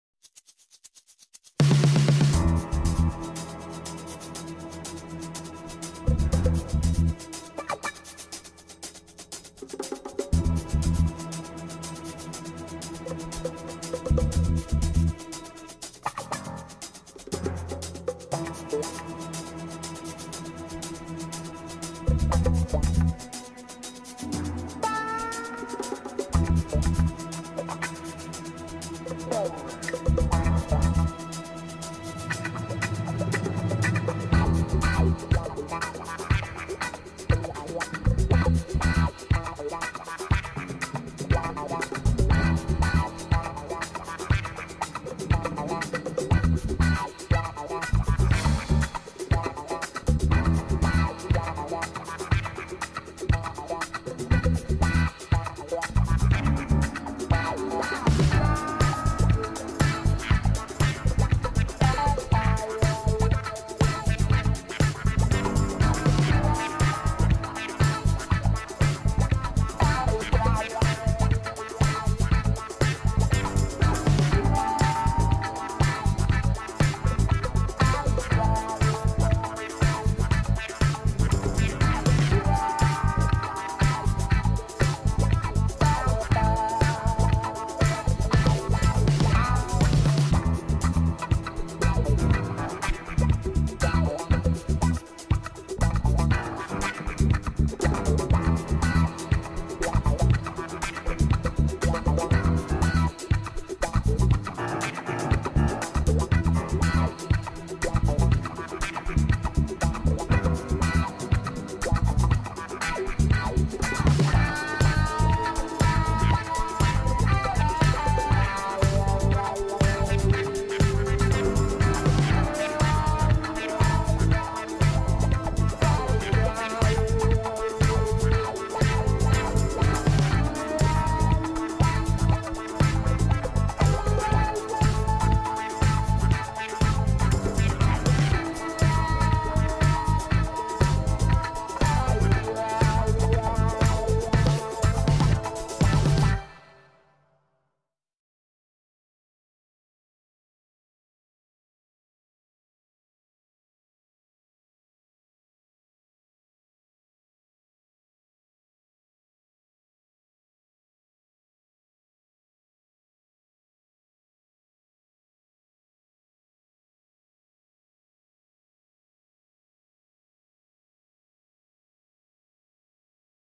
Sketching a ford Raptor pickup truck
Just tried a Ford Raptor futuristic version.. maybe electric version of Raptor... as its the first video. wanted to start with something muscular.